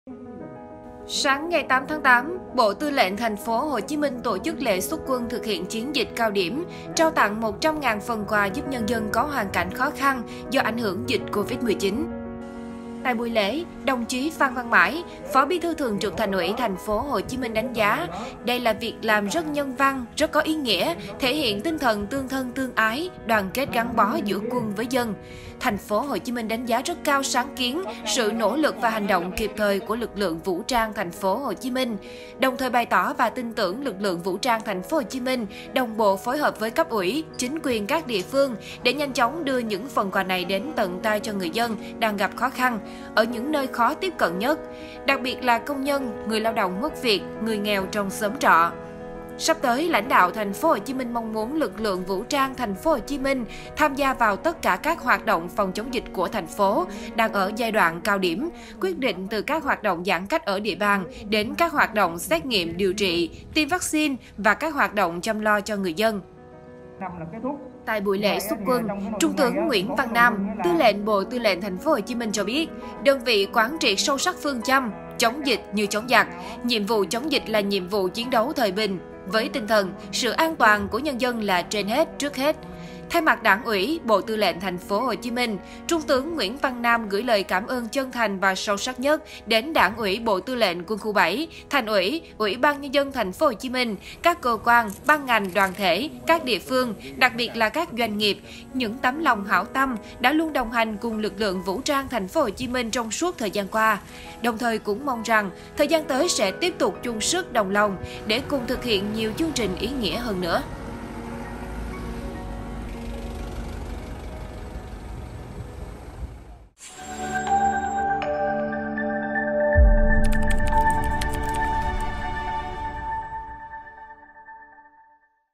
100.000 phần quà nghĩa tình chia sẻ khó khăn với người dân vượt qua dịch bệnh
Sáng 8-8, Bộ Tư lệnh TPHCM tổ chức Lễ xuất quân thực hiện chiến dịch cao điểm trao tặng 100.000 phần quà giúp nhân dân có hoàn cảnh khó khăn do ảnh hưởng dịch Covid-19.